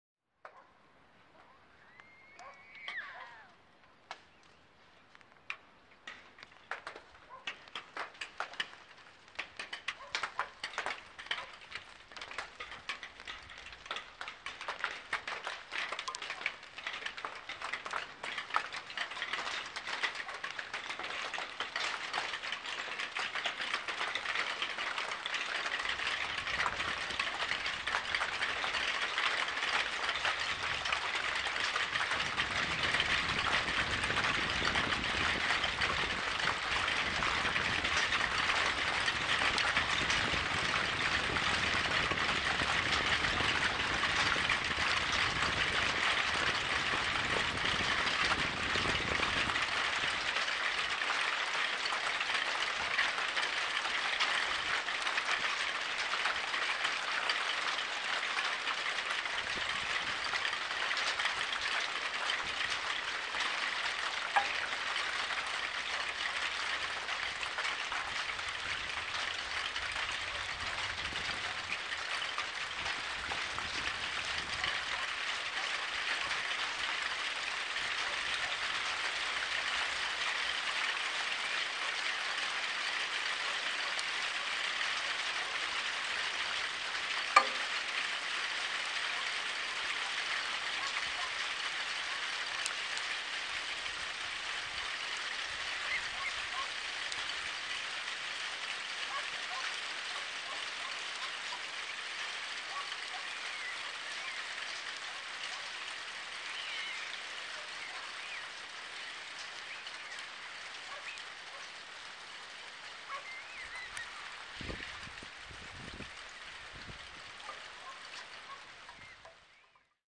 Hrajeme si s kroupami (až 3cm) v bouřce | hnízdo Bohuslavice u Trutnova
Čas: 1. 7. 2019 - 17:25